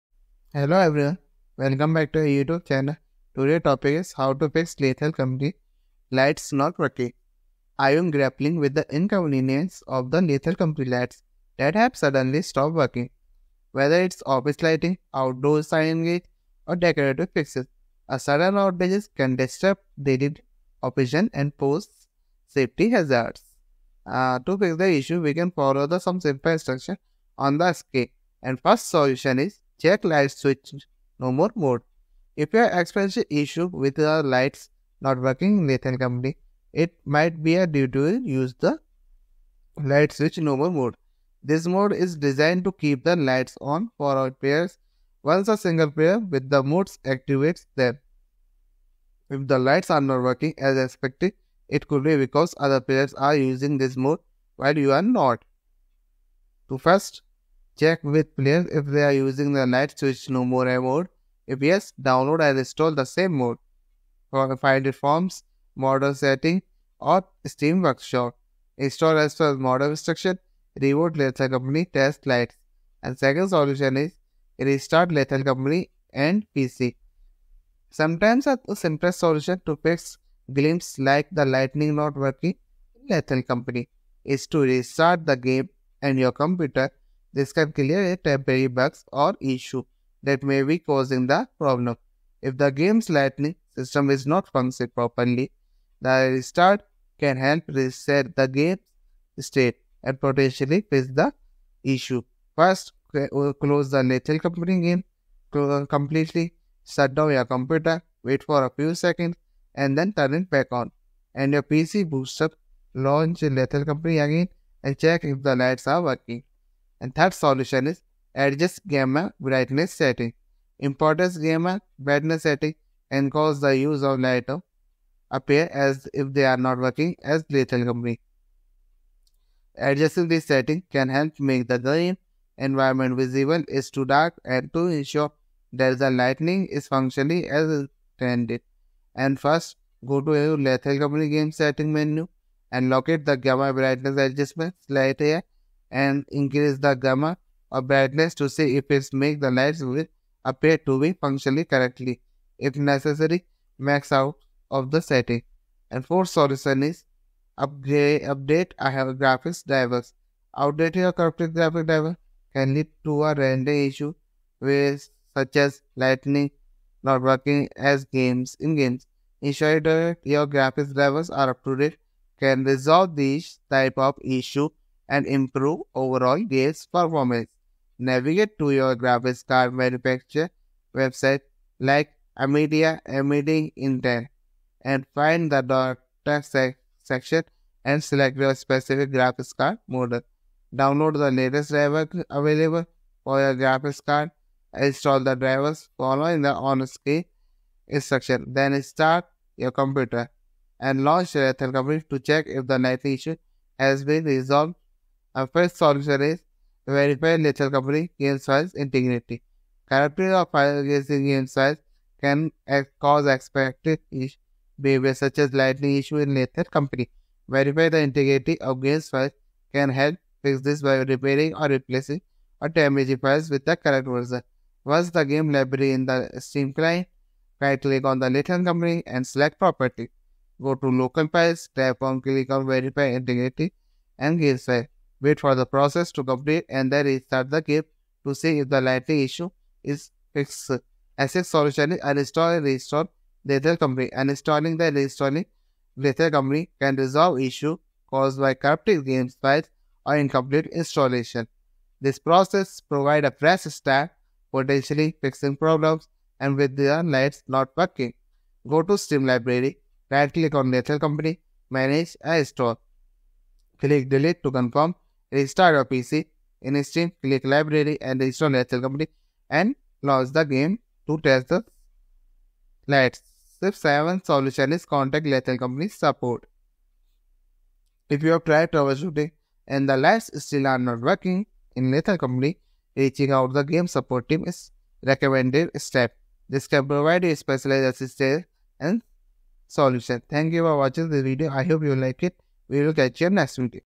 How To Fix Lethal Company Lights Not Working
Are you grappling with the inconvenience of lethal company lights that have suddenly stopped working?
Whether it’s office lighting, outdoor signage, or decorative fixtures, a sudden outage can disrupt daily operations and pose safety hazards.

00:00 Introduction
00:29 Solution 1: Check LightSwitchNoMore Mod
01:13 Solution 2: Restart Lethal Company And PC
01:58 Solution 3: Adjust Gamma/Brightness Settings
02:36 Solution 4: Update Graphics Drivers
03:26 Solution 5:Verify Lethal Company Game Files Integrity
04:02 Solution 6: Uninstall And Reinstall Lethal Company
04:34 Solution 7: Contact Lethal Company Support